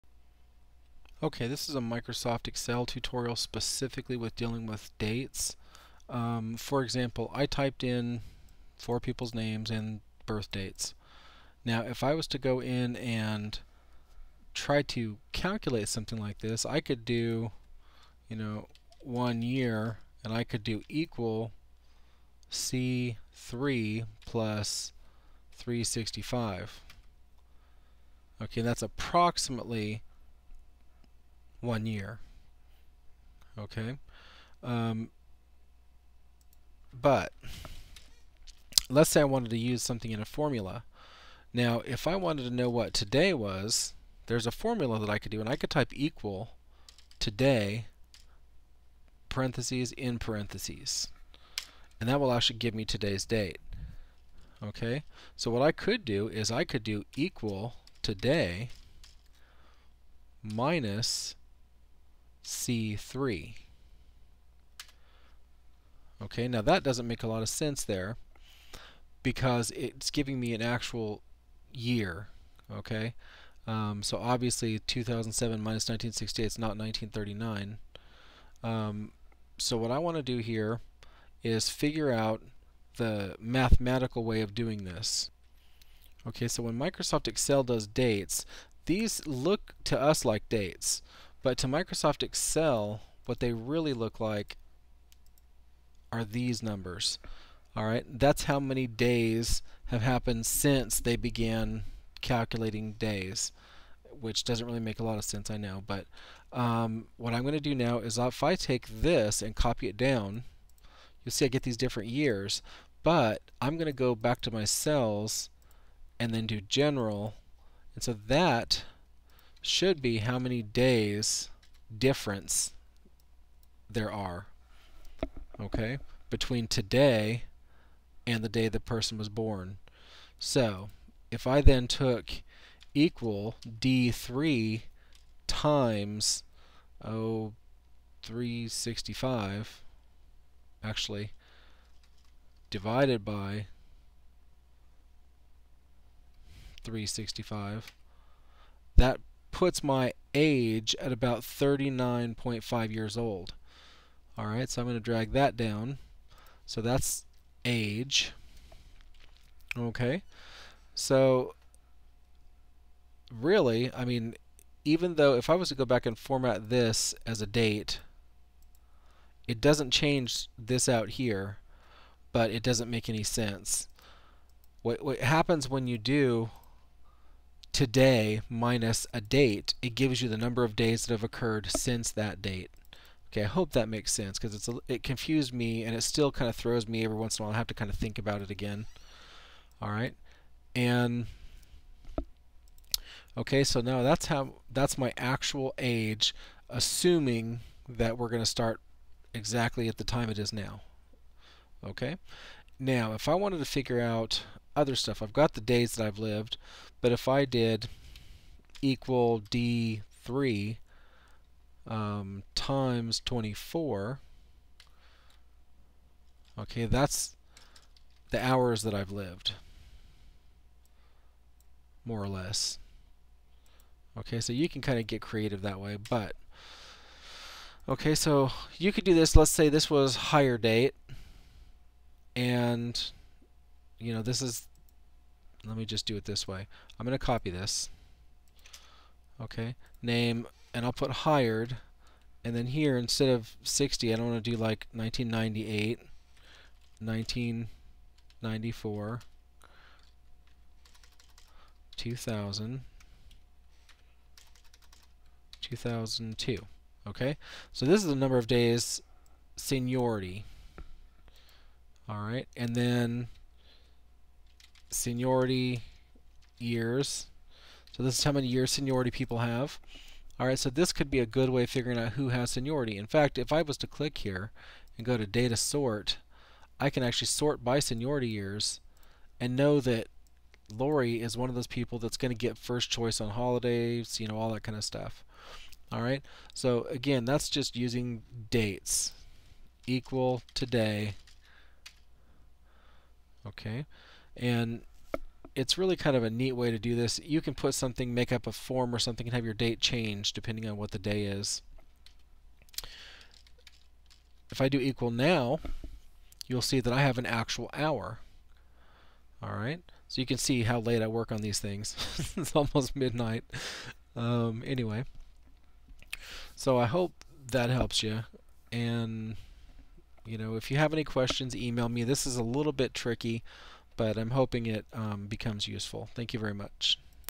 Microsoft Excel #08: Working with Dates
We will go over formatting dates and using dates in formulas.  We will calculate the number of days you've been alive, an employees seniority, etc.  We'll learn the "today()" and "now()" functions.